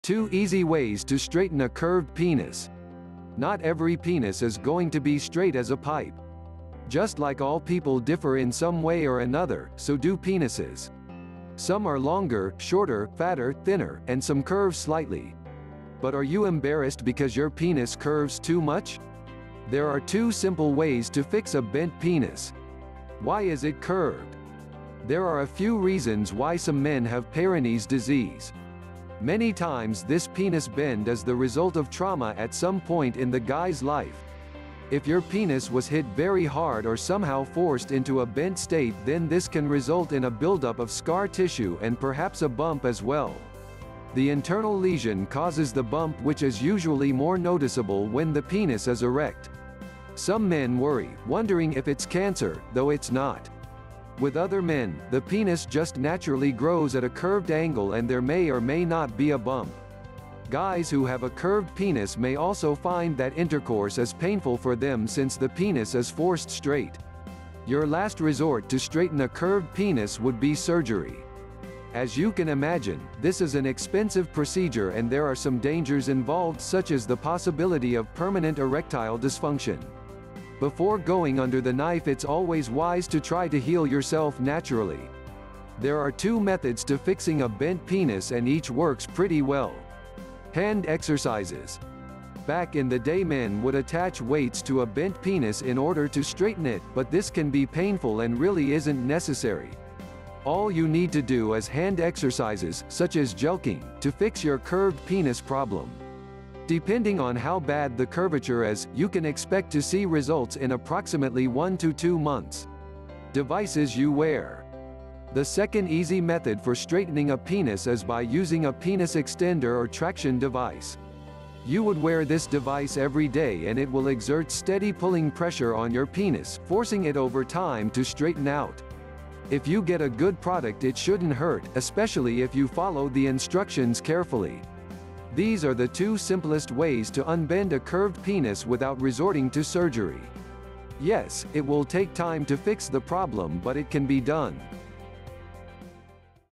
2 Easy Ways To Straighten A Curved Penis
Not every penis is going to be straight as a pipe. Just like all people differ in some way or another, so do penises. Some are longer, shorter, fatter, thinner, and some curve slightly. But are you embarrassed because your penis curves too much? There are two simple ways to fix a bent penis.
Why Is It Curved?
There are a few reasons why some men have Peyronie’s disease (a curved penis). Many times this penis bend is the result of trauma at some point in the guy’s life. If your penis was hit very hard or somehow forced into a bent state then this can result in a buildup of scar tissue and perhaps a bump as well. The internal lesion causes the bump which is usually more noticeable when the penis is erect. Some men worry, wondering if it’s cancer, though it’s not. With other men, the penis just naturally grows at a curved angle and there may or may not be a bump. Guys who have a curved penis may also find that intercourse is painful for them since the penis is forced straight.
Your last resort to straighten a curved penis would be surgery. As you can imagine, this is an expensive procedure and there are some dangers involved such as the possibility of permanent erectile dysfunction. Before going under the knife it’s always wise to try to heal yourself naturally. There are two methods to fixing a bent penis and each works pretty well.